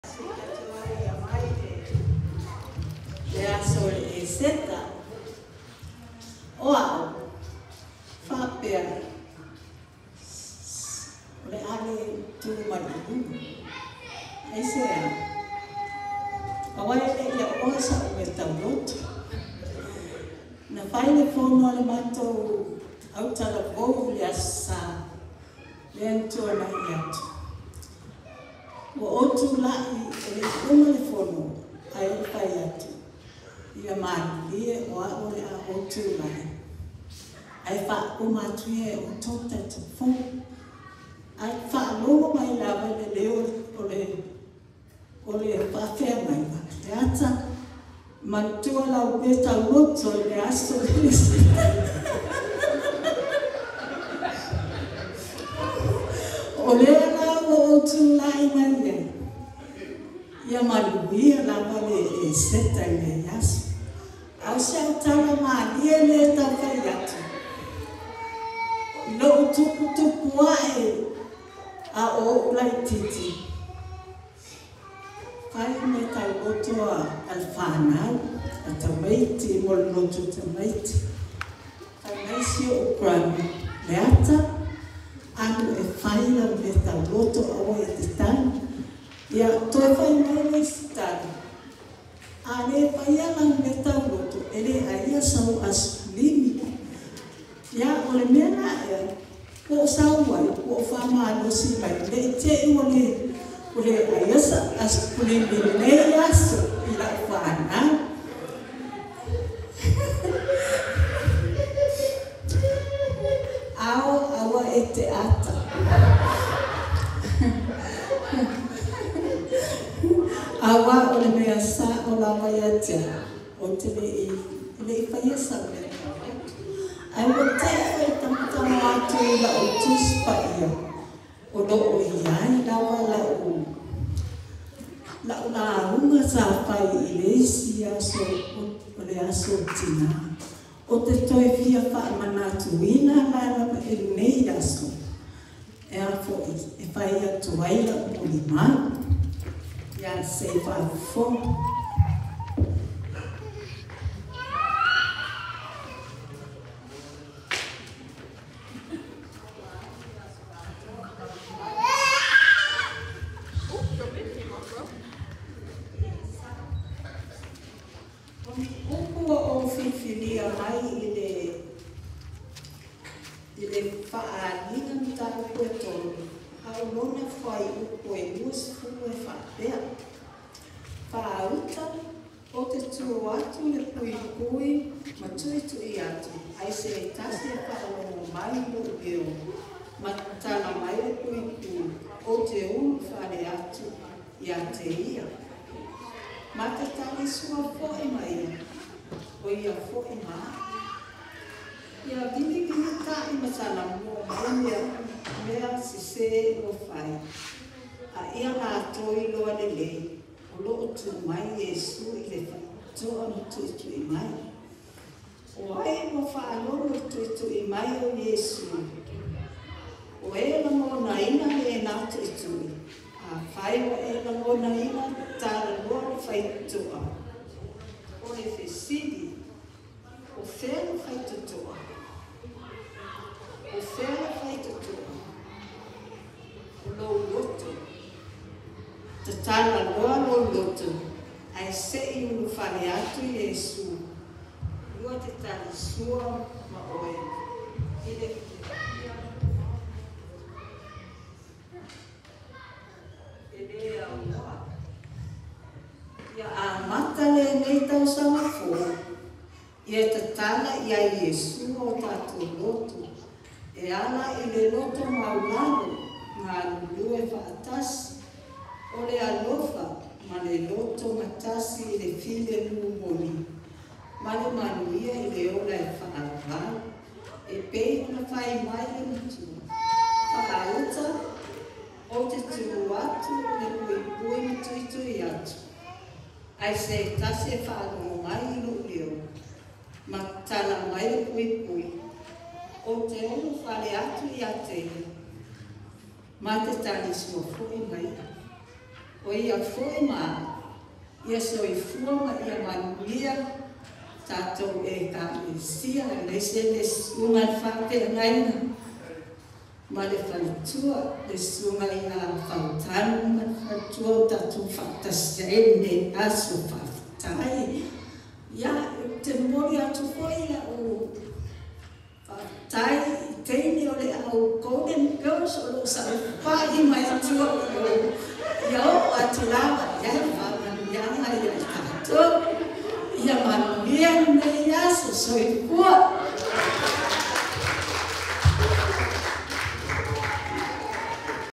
PFCC Easter 2023 Tupu Family
Long Beach CA Peteli Fou Christian Church Online Easter Service April 9, 2023 Tupu Family